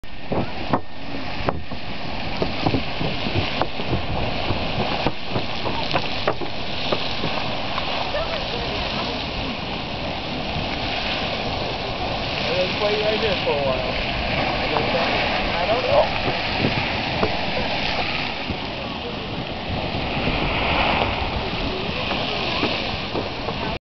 Yellowstone geysers Wyoming 10 2004
several geysers erupting in Yellowstone National Park, Wyoming